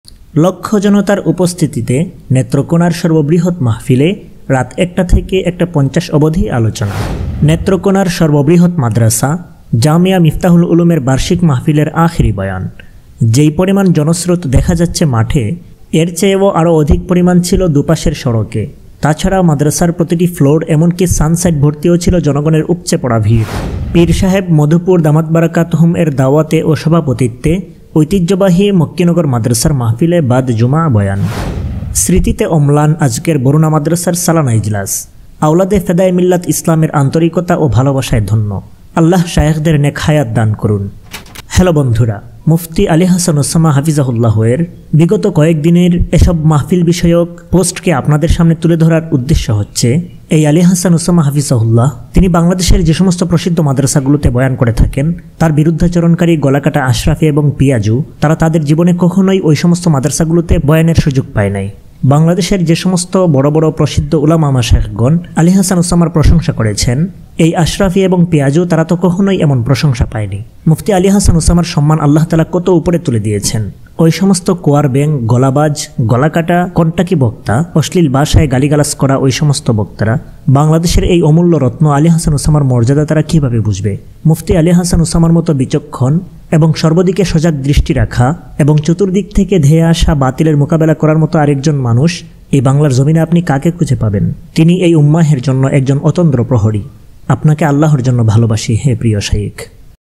আলী হাসান উসামা যেসব বড় মাদ্রাসায় দাওয়াত পায়, আশ্রাফী ও মিয়াজী কখনো পায় না | new bangla waz mahfil
শুয়াইব আহমদ আশ্রাফী shoaib ahmad ashrafi | আব্দুল কাইয়ুম মিয়াজী abdul kaiyum miaji | আলী হাসান উসামা ali hasan osama new waz